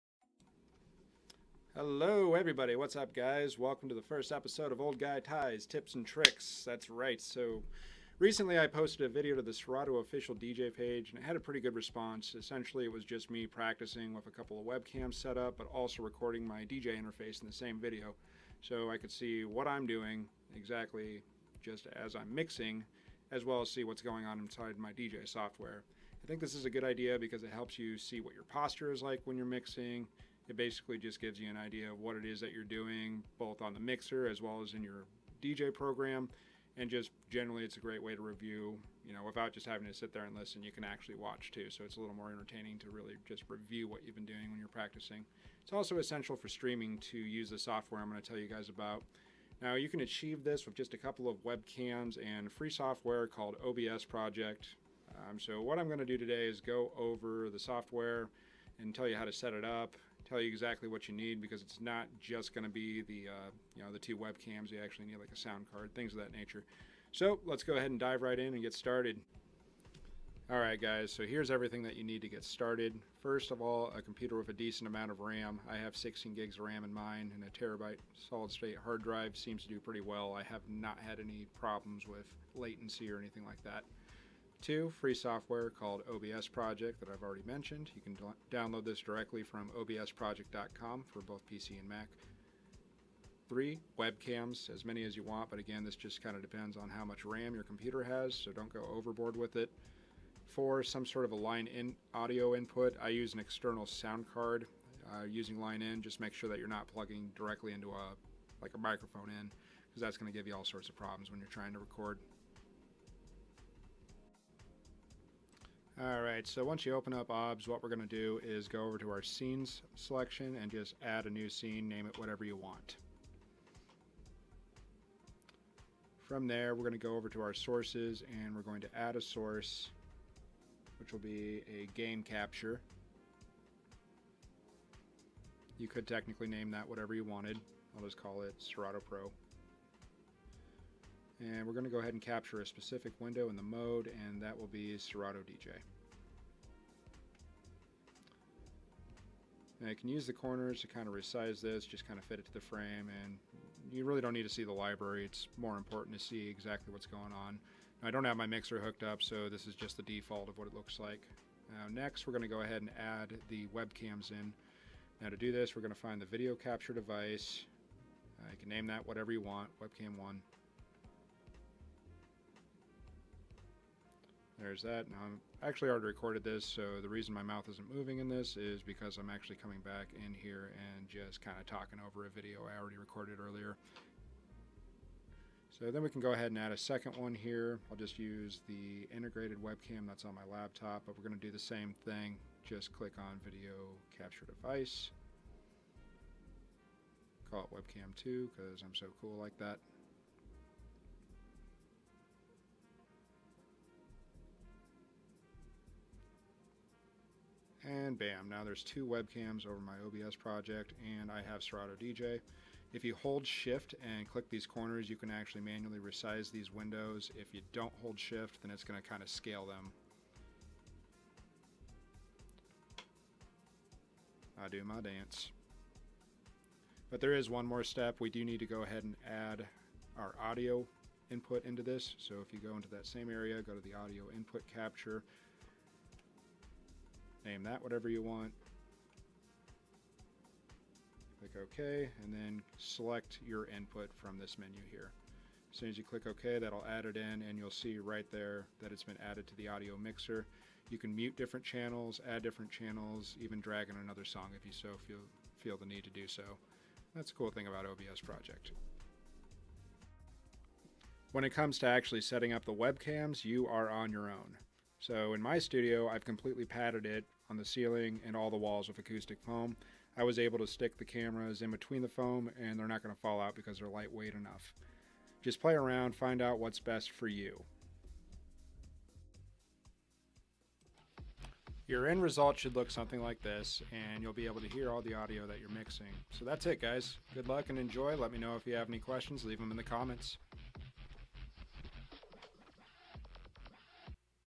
Recording Your DJ Practice with OBS Project
This is a tutorial on how to set up OBS to record your DJ software and webcams in the same video.